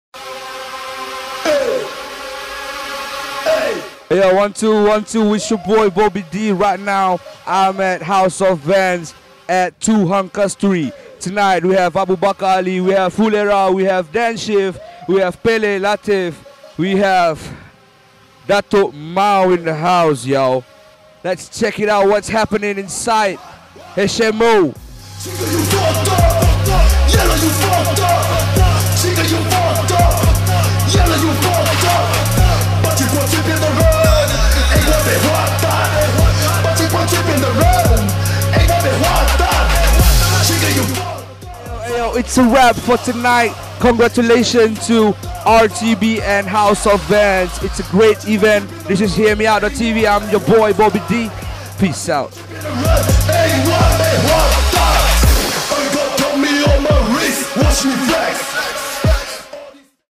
House Of Vans 2018
Heameout.tv presents to you event highlights throwback Raising The Bar at House of Vans.

Venue : 2 Hang Kasturi, KL
DATE :7th Sept 2018